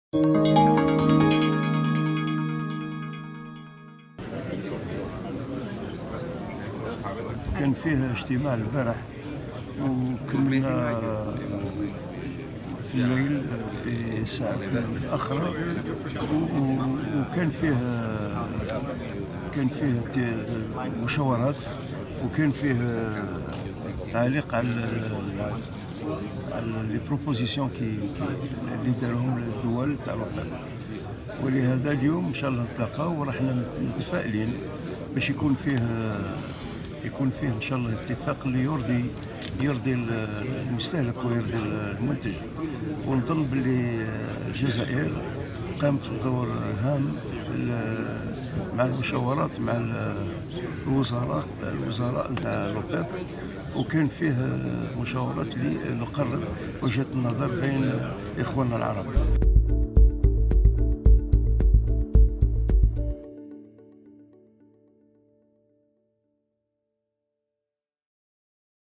قيطوني: الجزائر تعمل على تقريب وجهات نظر دول الأوبك
فيينا (وأج) - أكد وزير الطاقة مصطفى قيطوني أن الجزائر تعمل على تقريب وجهات النظر بين دول منظمة الأوبك،  و هذا خلال مشاركته في أشغال الندوة الوزارية الـ 174 لمنظمة الأوبك بفيينا.